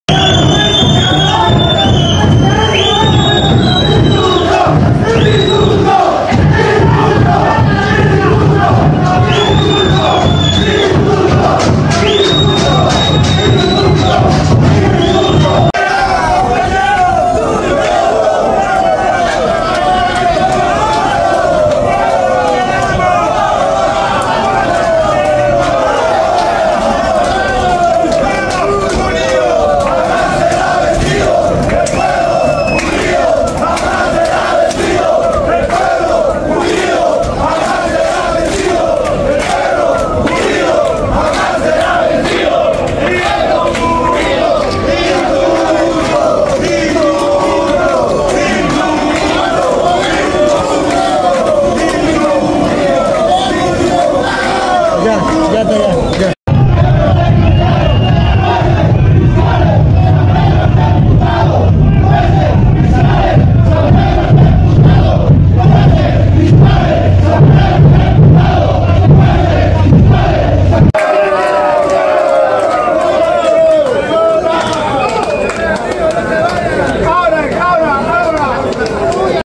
PRIVADOS DE LIBERTAD EXIGEN EN DIFERENTES CÁRCELES QUE SE APRUEBE LA LEY DE INDULTO
En las cárceles de San Pedro de La Paz, San Sebastián de Cochabamba y otras los privados de libertada protestan exigiendo la aprobación de la Ley de Indulto.